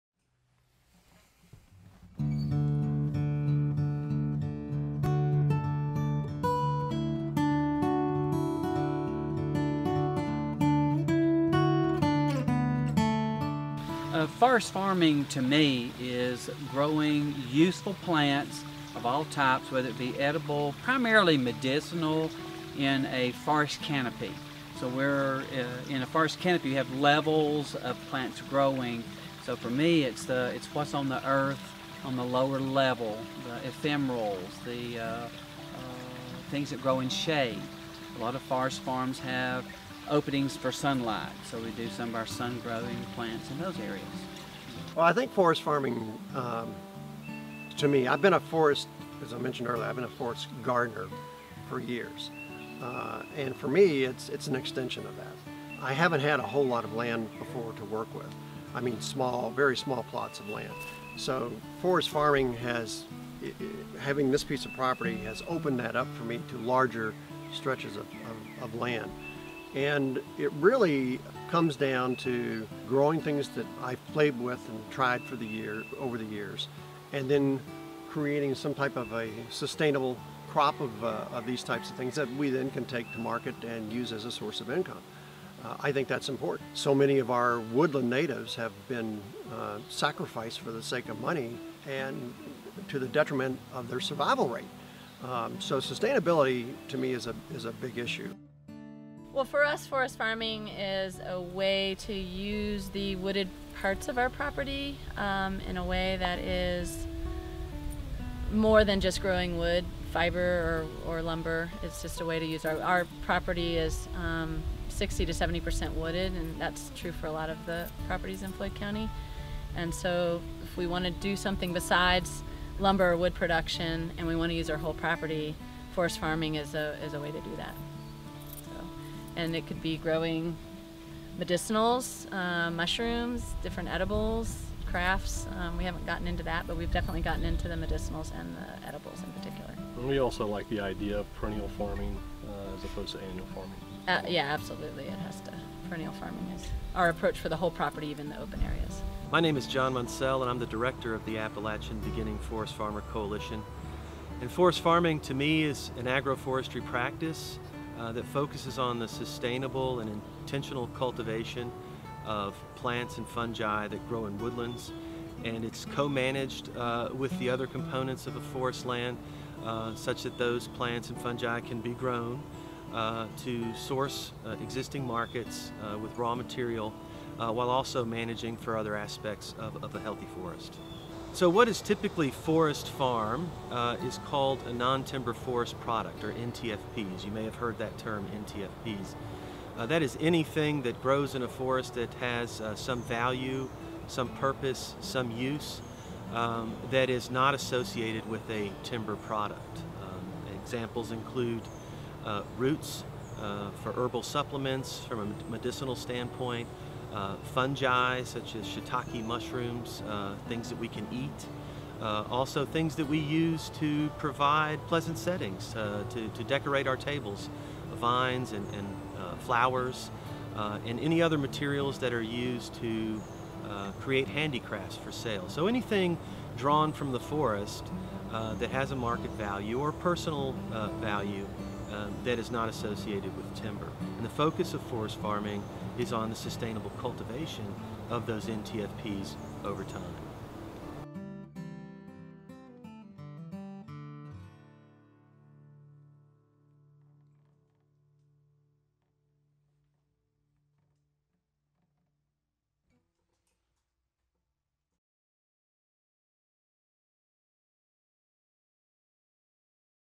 What is forest farming?
Forest farming is an agroforestry practice which cultivates medicinal and edible crops, decorative and craft materials and more under a forest canopy that is managed to provide ideal shade levels and habitat.

Forest farming on a larger scale supplies marketable crops, materials and products made from non timber forest products to community, local, regional and online marketplaces as well as internationally.

Forest farming also provides several opportunities to land owners, families and communities to benefit while managing, protecting and conserving the forests for the future.

Learn more about forest farming and join our ever growing community on appalachianforestfarmers.org!